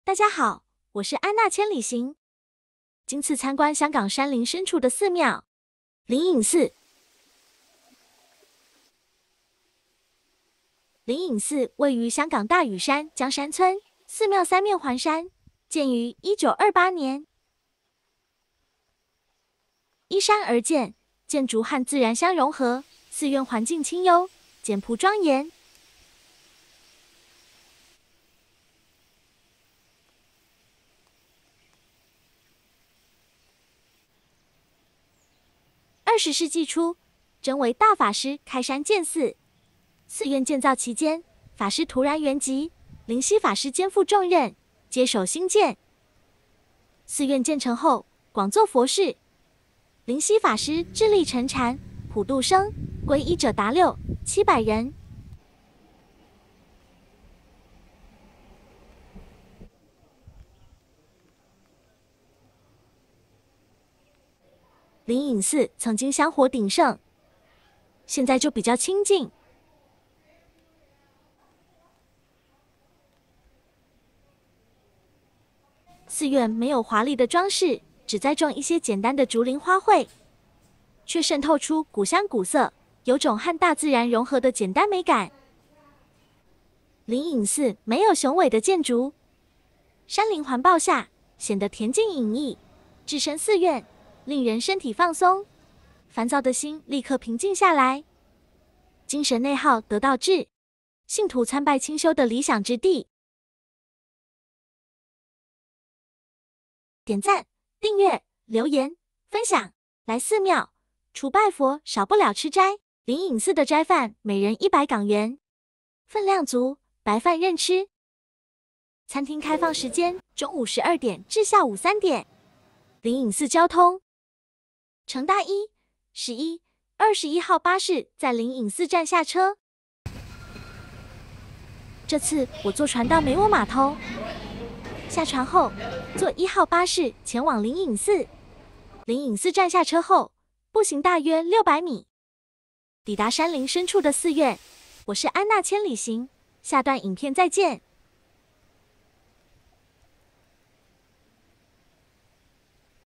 山林深處的簡美寺院, 治癒精神內耗 | 遠離城市煩囂, 禪修放鬆之地 | 寺廟吃齋飯#香港好去處#拍攝好地方
坐落在香港大嶼山薑山村的靈隱寺, 有90多年的歷史。相對其它富麗堂皇的寺廟,  靈隱寺雖然不能與其媲美。但在環山群抱下, 簡單建築佈局的靈隱寺卻有一種與自然環境相融合的素雅之美。